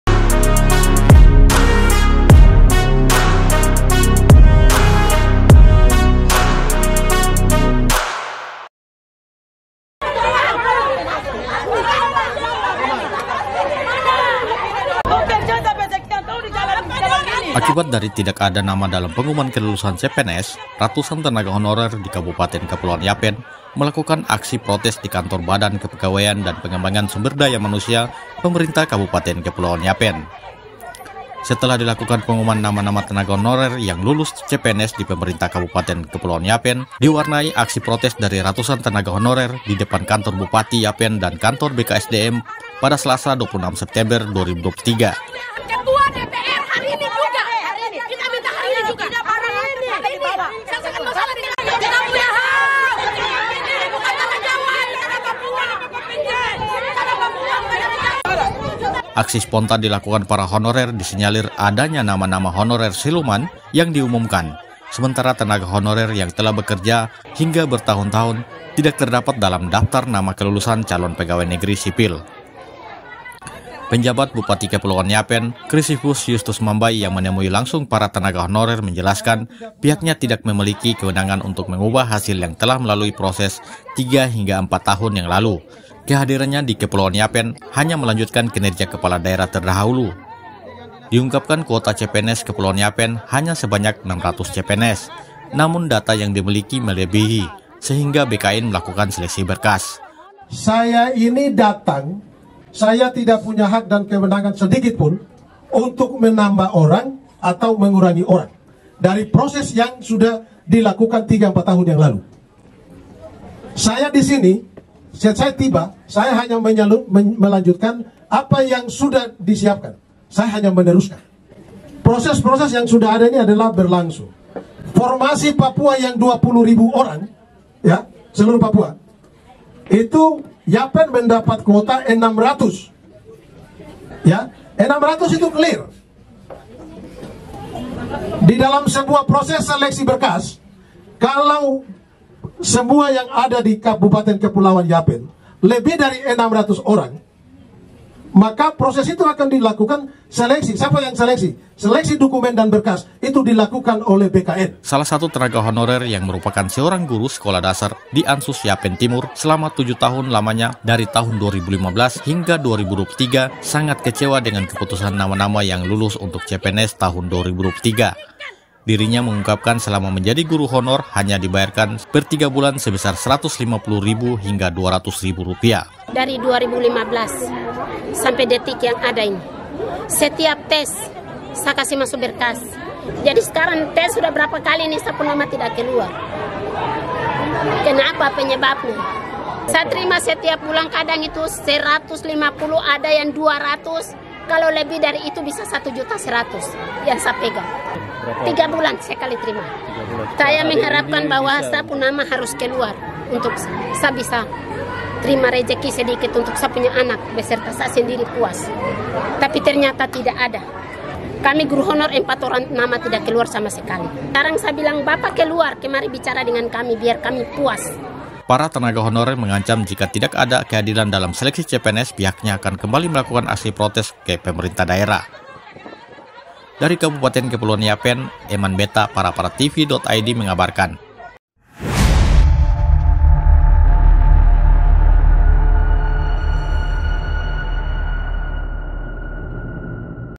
Protes Ratusan Tenaga Honorer Dengan Hasil CPNS 2023 Pemkab Kepulauan Yapen
Paraparatv.id | VIDEO | Akibat dari tidak ada nama dalam pengumuman kelulusan CPNS  ratusan tenaga honorer di kabupaten Kepulauan Yapen melakukan aksi protes di kantor badan kepegawaian dan pengembangan sumber daya manusia, pemerintah kabupaten Kepulauan Yapen.

Setelah di lakukan pengumuman nama-nama tenaga honorer yang lulus CPNS di pemerintahan daerah kabupaten Kepulauan Yapen  di warnai aksi protes dari ratusan tenaga honorer di depan kantor Bupati Yapen dan kantor BKSDM,  pada selasa 26 September 2023.